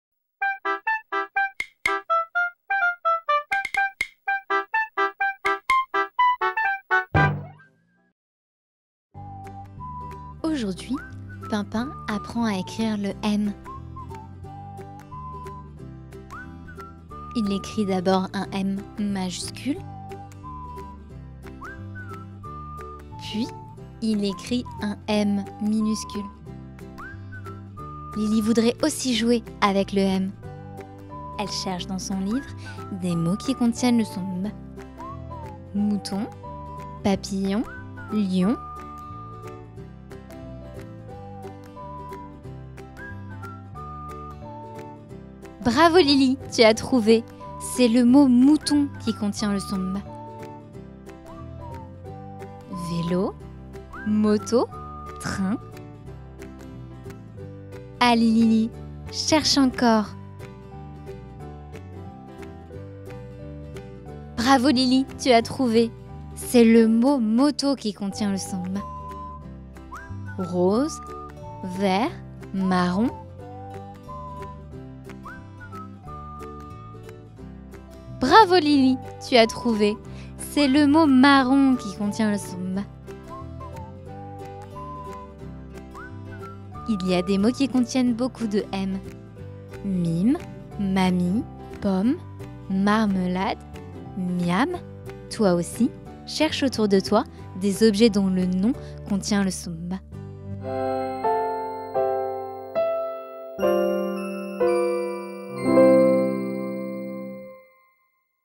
Les lettres de l'alphabet - Apprendre à écrire le M avec Pinpin et Lili
Pour apprendre à écrire le M en majuscule et en cursive, et pour s'amuser à reconnaître le M dans des mots, voici une vidéo destinée aux enfants, avec les petits héros Pinpin et Lili.

Pinpin apprend à écrire le M, puis Lili joue avec des imagiers pour retrouver le son M dans des mots qu'elle connaît.

Par le biais du jeu, des jolies illustrations et du rythme tout en douceur, l'enfant continue de se familiariser paisiblement avec les lettres qu'il apprend à l'école. Une bonne manière de compléter les apprentissages qui sont menés par la maîtresse et de les consolider. Attendez que votre enfant les ait découvertes en classe, pour éviter toute confusion.

Retrouvez toutes les vidéos de Pinpin et Lili sur leur chaîne Youtube

Retrouvez toutes nos vidéos d'activités manuelles pour enfants (coloriages, apprendre à dessiner, bricolages, recettes)

Remerciements à Sophie Cottin, institutrice (Ecole Saint Joseph de Bellevue - Meudon) pour ses précieux conseils.